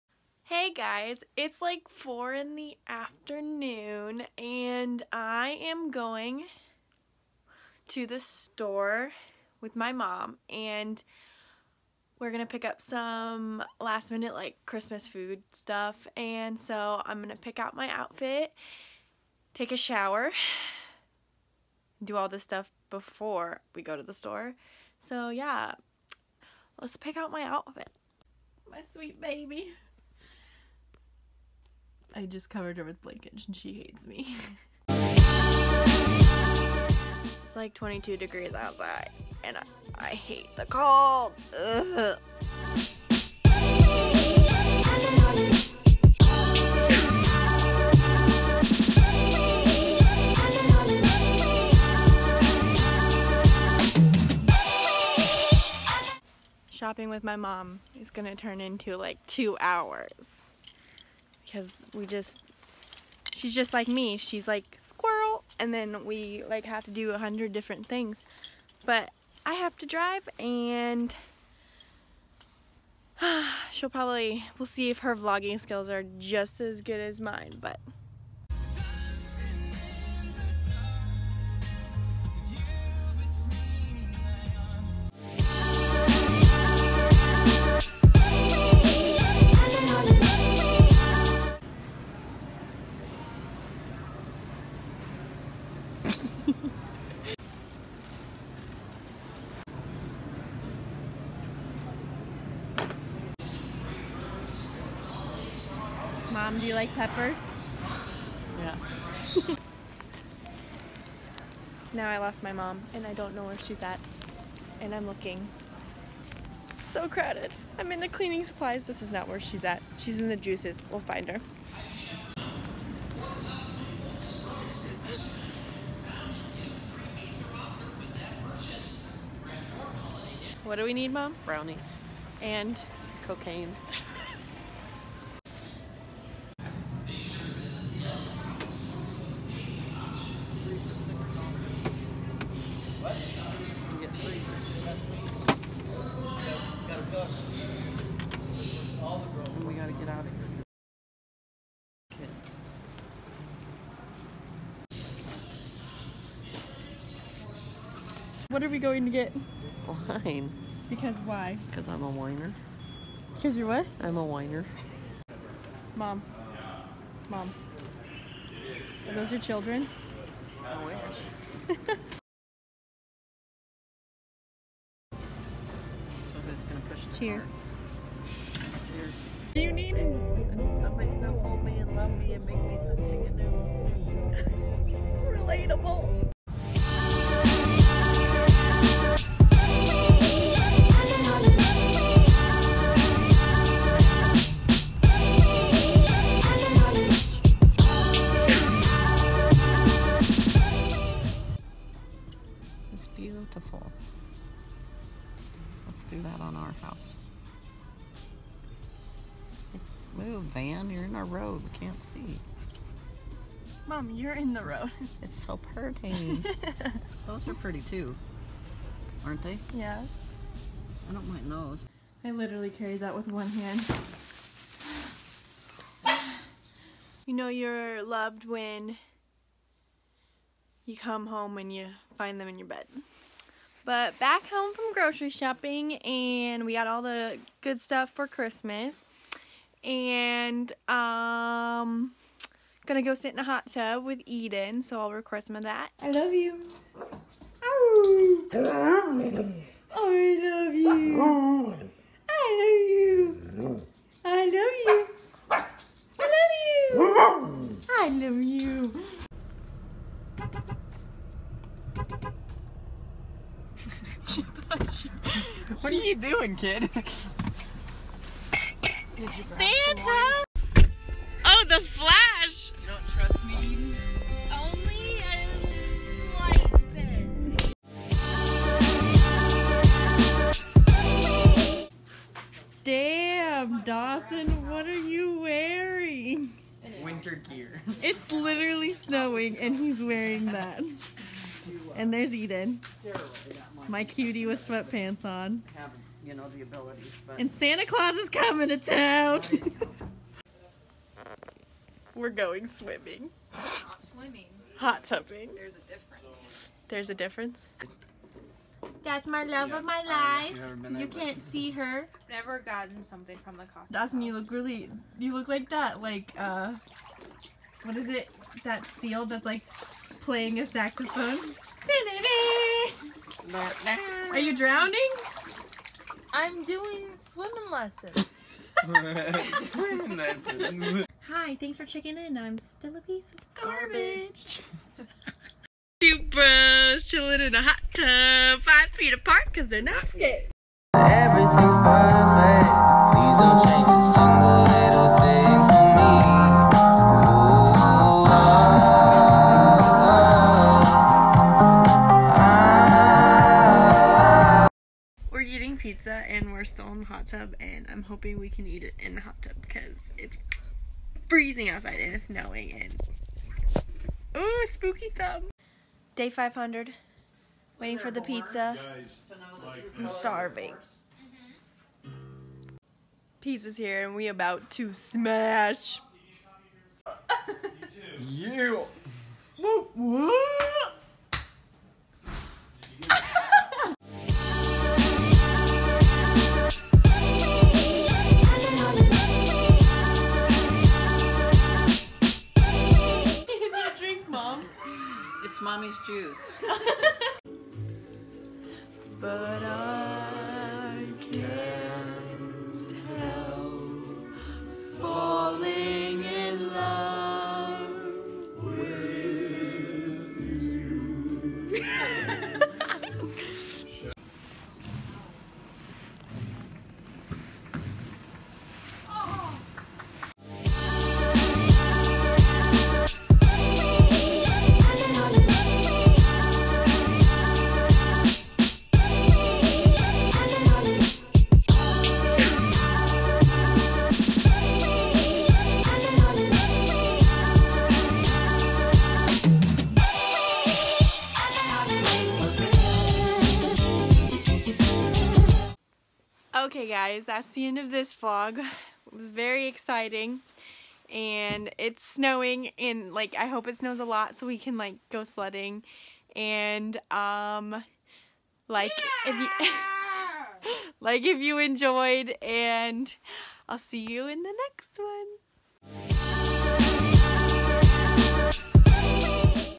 VLOG 5 | HYVEE & HOT TUBBING
Grocery store and hot tubbing. IT SNOWED!!

Follow me on:
Pinterest: @CeyennaB or Ceyenna Jyade